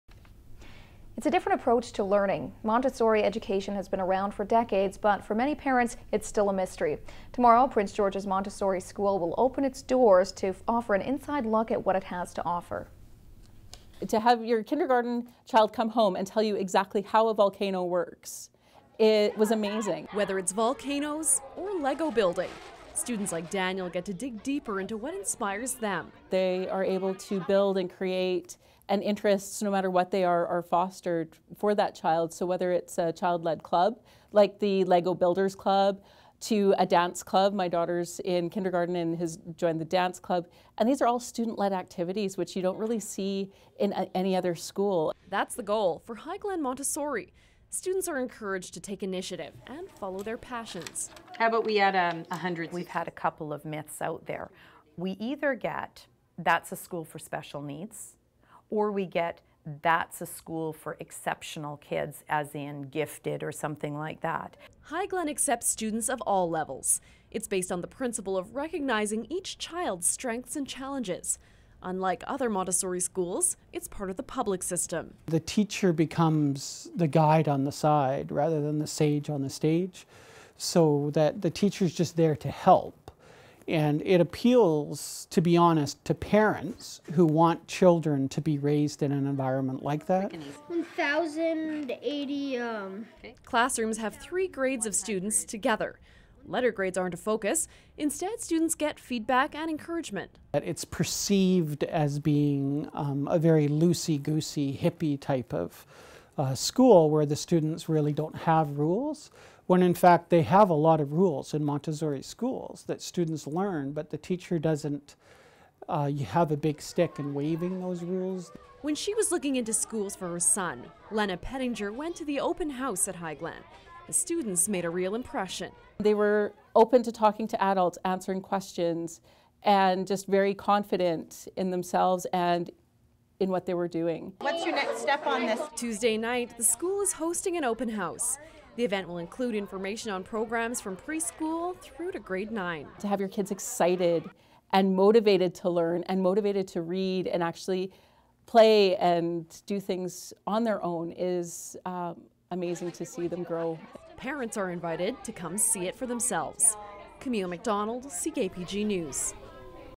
Highglen Hosts Open House
It's a different approach to learning. Montessori education has been around for decades, but for many parents, it's still a mystery. Tomorrow, Prince George's Montessori school will open its doors, to offer an inside look at what it has to offer.